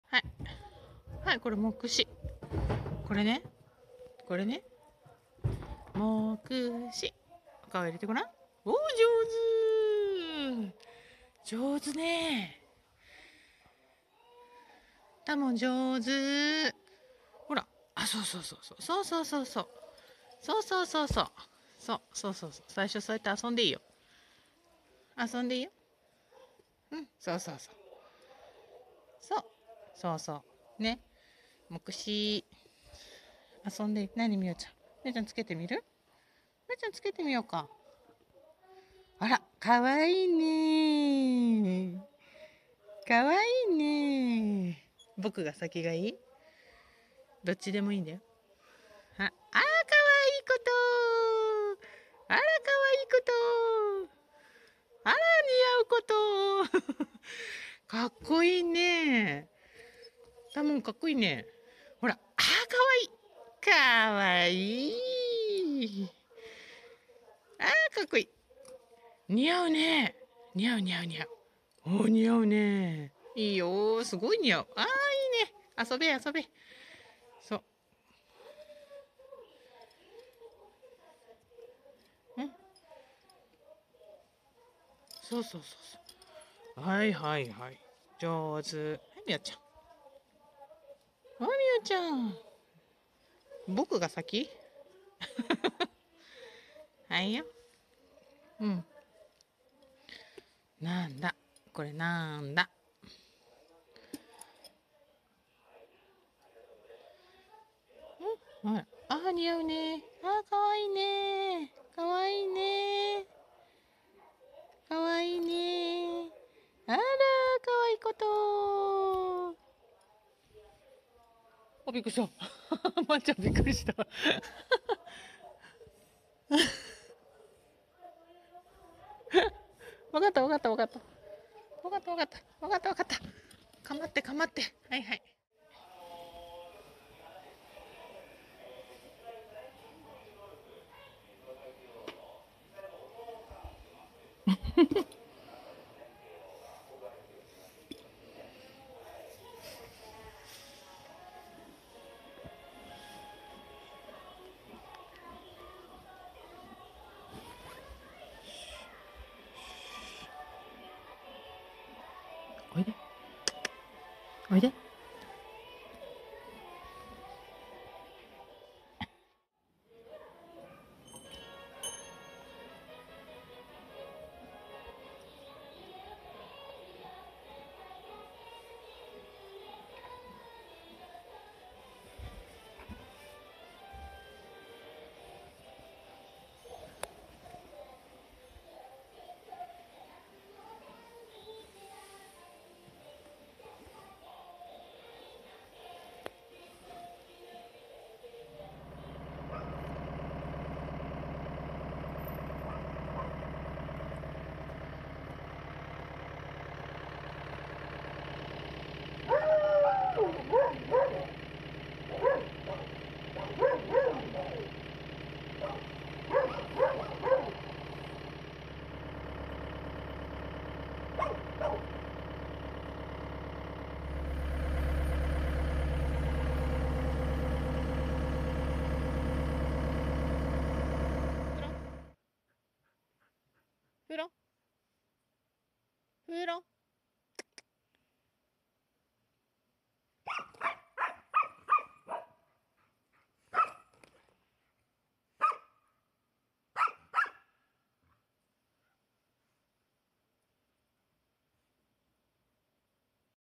生まれて初めてのモクシをつける澪と多聞・ふろの掘った大穴埋め戻し
今日は澪と多聞の初モクシです。
おだてておだてておだてまくって何とか装着できました。

そして、ふろの掘った大穴の埋め戻しです。
どんな穴を掘ってきたかはこちらをご覧ください。